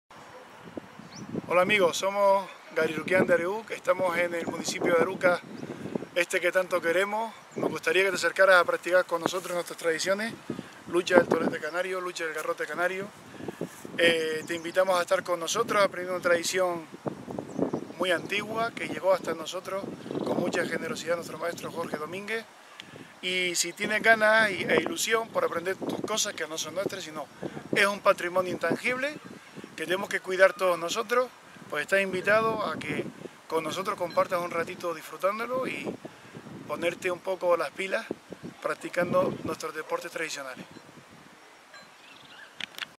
Practica Lucha del Garrote y Tolete Canario con nosotros
Somos el equipo Gariruquian Arehuc y practicamos deportes autóctonos como la lucha del garrote canario y la lucha del tolete canario.
Te invitamos a participar y a aprender con nosotros.